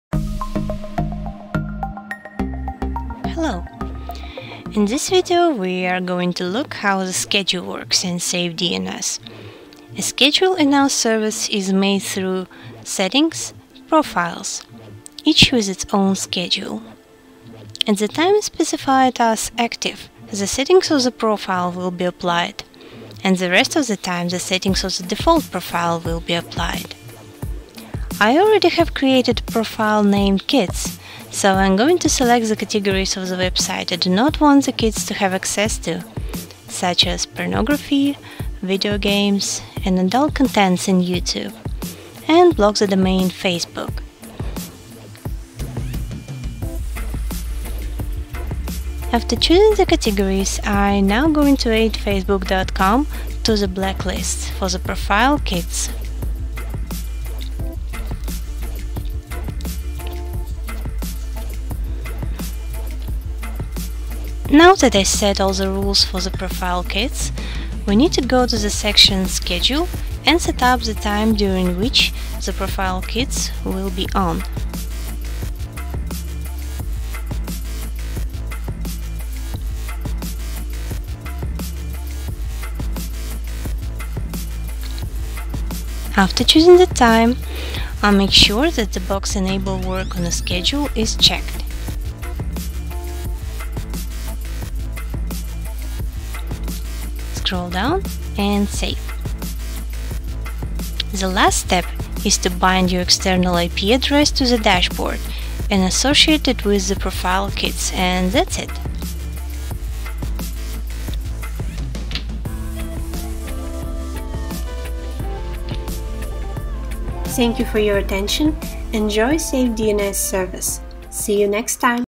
How to set up filtering schedule with SafeDNS service
This video guide describes how to set up and manage a web filtering schedule.

Using this option you can limit internet access for certain filtering profiles during specific time periods and choose when and which filtering policy will be applied. Try it! It's easy!

Still have questions? Read the schedule guide

Make sure to comment and give us your thoughts on our web filtering solutions.